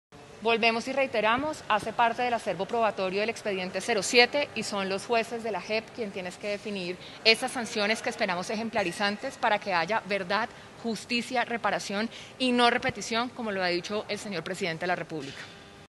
"Testimonios de niños y niñas víctimas de las Farc confirman que sí hubo política para reclutarlos”
"Testimonios de niños y niñas víctimas de las Farc confirman que sí hubo política sistemática para reclutarlos”